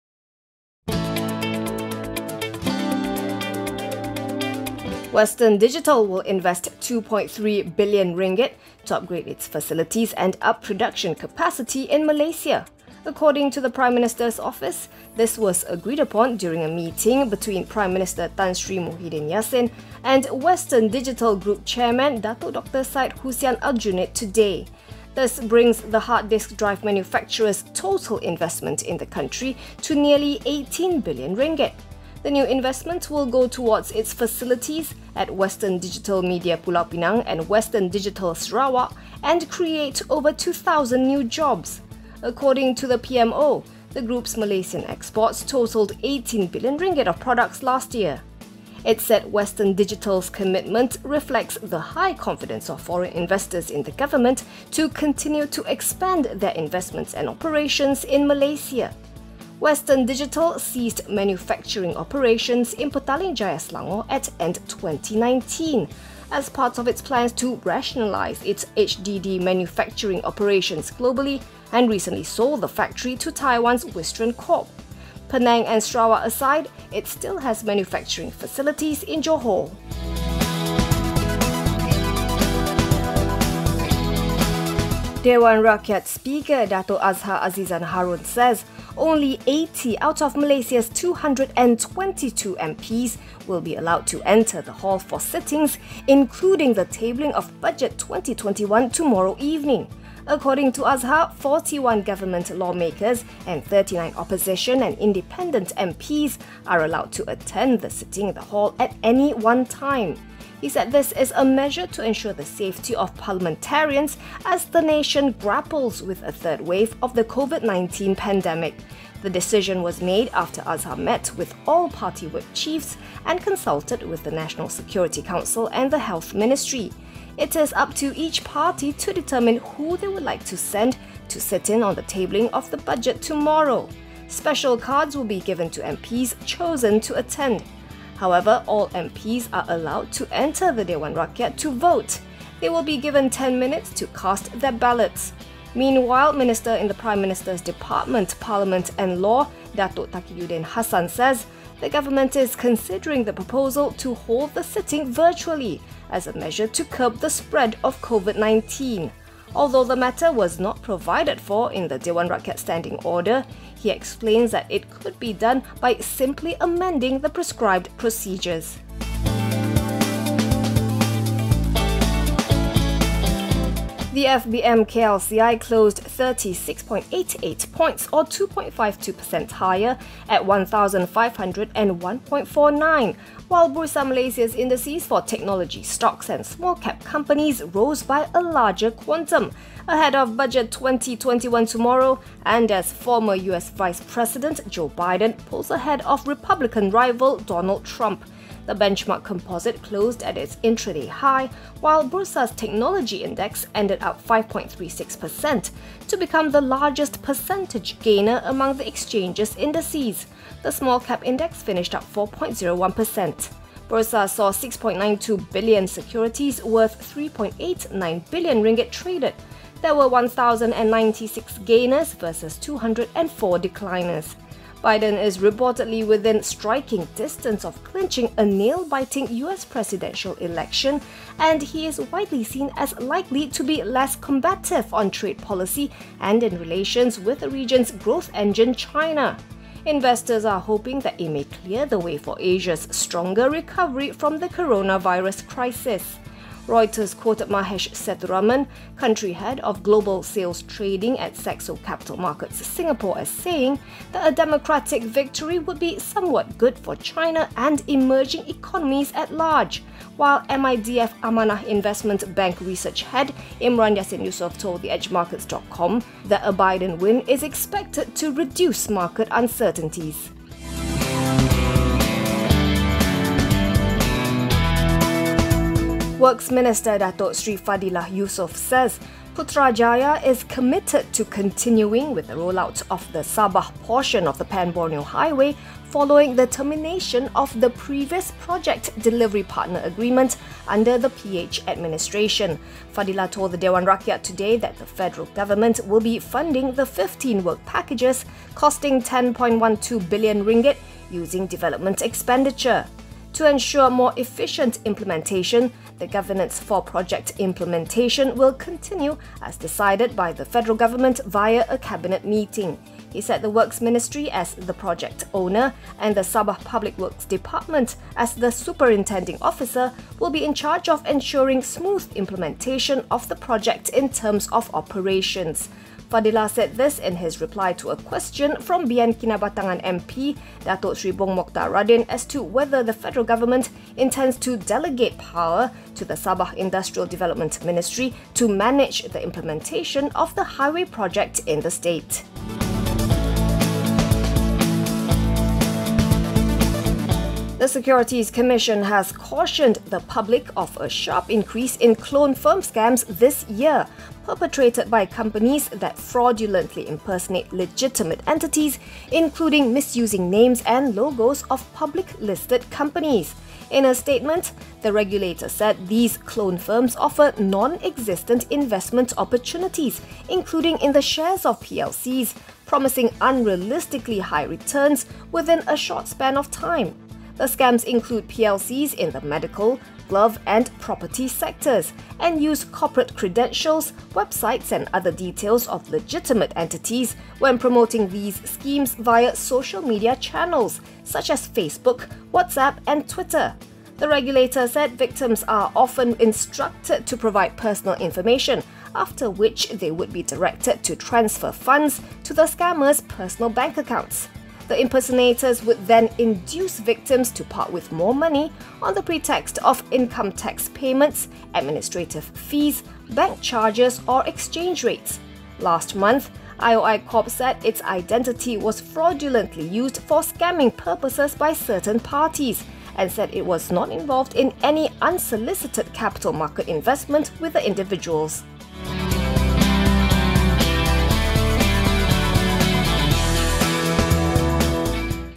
EVENING 5: Western Digital pledges RM2.3b investments
In today’s edition of Evening 5, Western Digital will invest RM2.3 billion to upgrade its facilities in Malaysia. Meanwhile, only 80 MPs will be allowed to enter the Dewan Rakyat for sittings, including the tabling of Budget 2021 tomorrow.